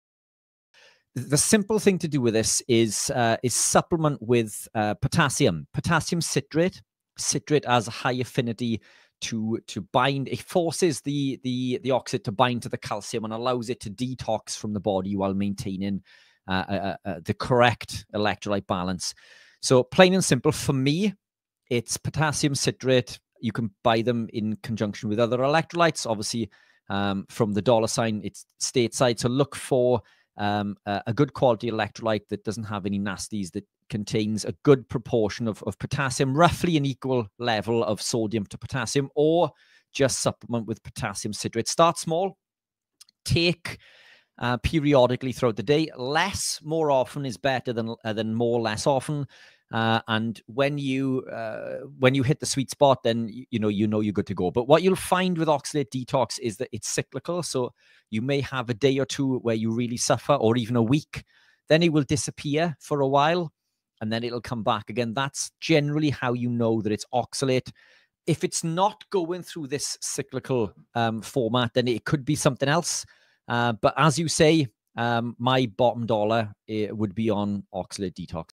Oxalate Detox: Best Method with Potassium Citrate Explained
Potassium Citrate & Oxalate Detox 🧪
When it comes to oxalate detox, I recommend potassium citrate. Why? Because citrate has a strong binding affinity. It helps oxalates bind to calcium so they can be excreted, while also supporting electrolyte balance.
Start small. Spread your intake throughout the day. It’s a simple but powerful way to support your body’s natural detox process.

👉 Have you tried potassium citrate for oxalate detox?